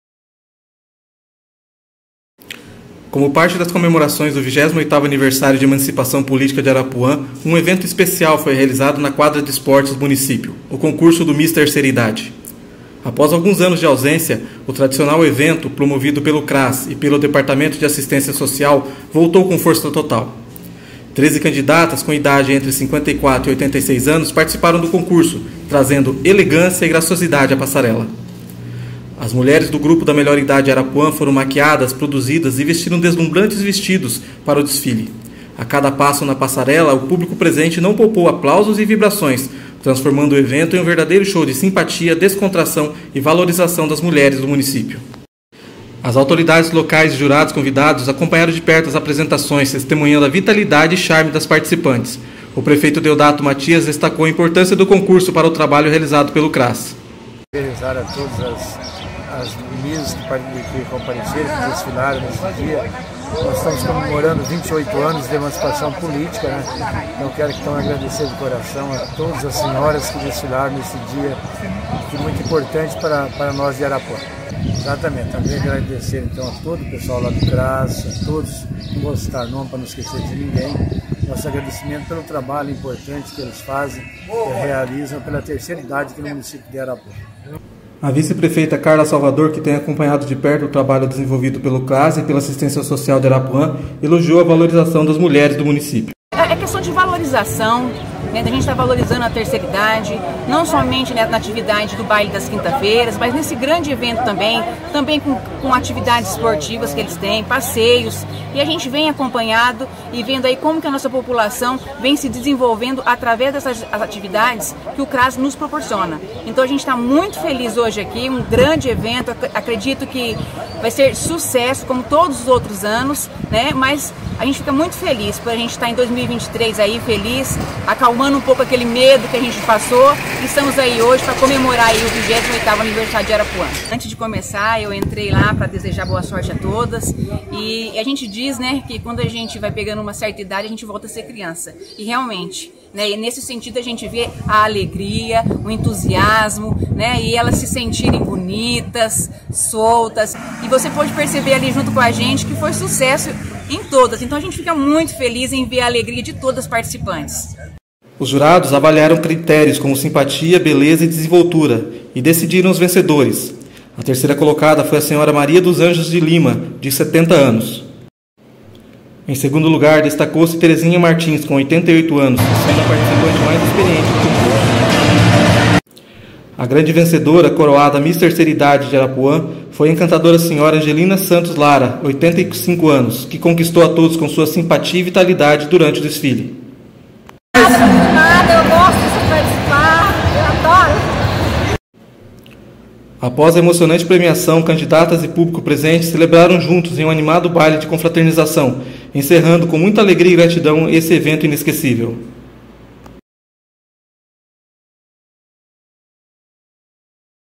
Arapuã elege Miss Terceira Idade
O município de Arapuã retomou a realização do tradicional concurso da Miss Terceira Idade. 13 candidatas participaram do evento que abriu as comemorações do aniversário de Arapuã